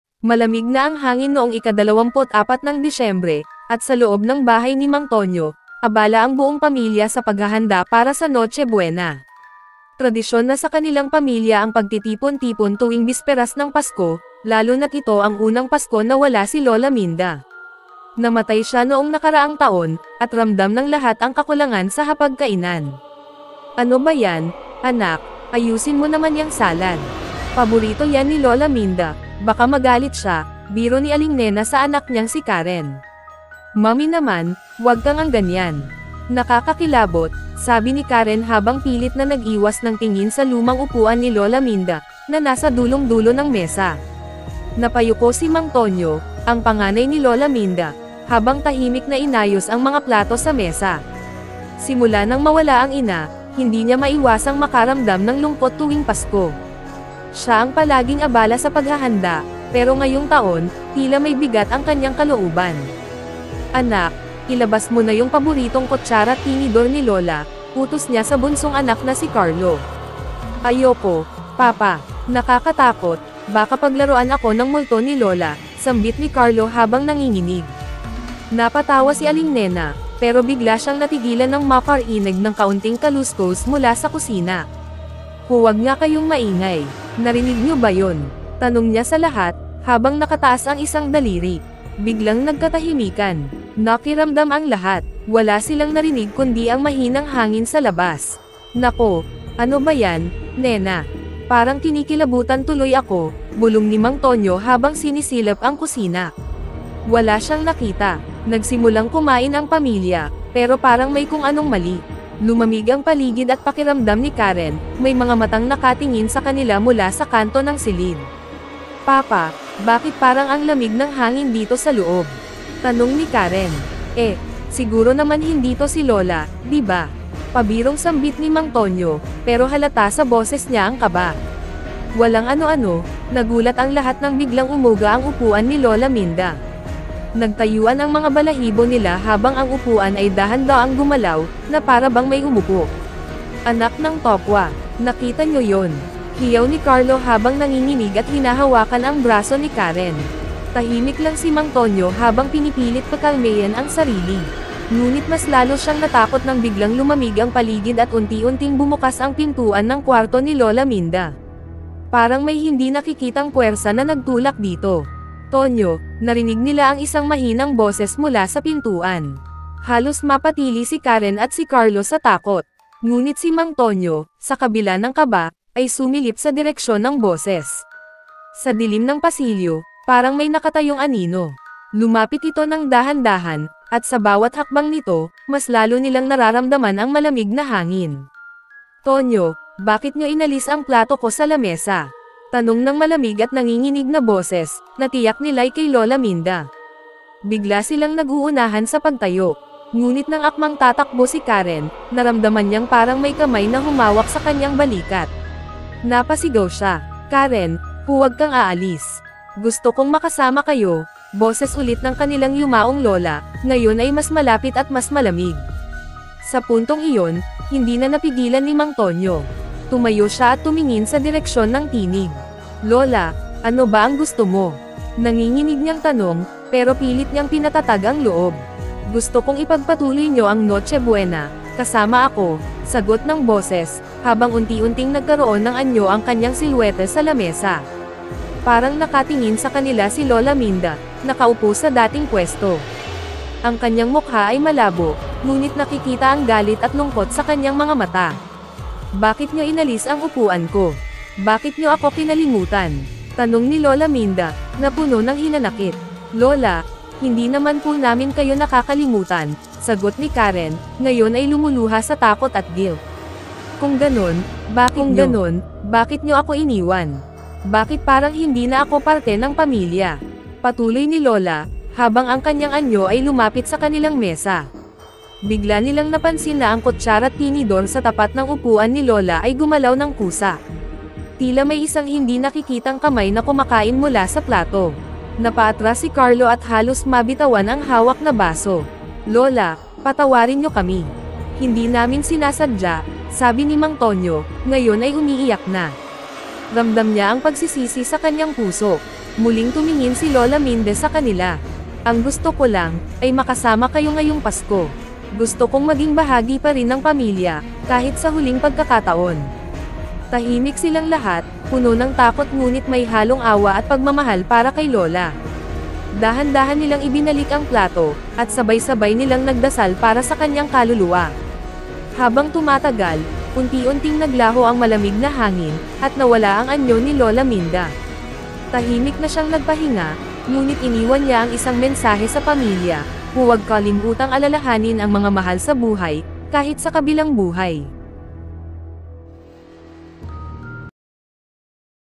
Ang Balik ni Lola Minda: Isang Nakakatakot na Pasko | Filipino Horror Komik Story
Sa unang Pasko na wala si Lola Minda, hindi inaasahan ng pamilya na babalik siya upang muling makasama sila. Ngunit hindi ito ang reunion na kanilang inaasahan! Tunghayan ang kwento ng isang pamilyang dinadalaw ng kaluluwa ni Lola sa isang nakakatakot at misteryosong Noche Buena. Makikita natin ang kanilang takot, pagsisisi, at pag-ibig para sa yumaong matriarka ng pamilya.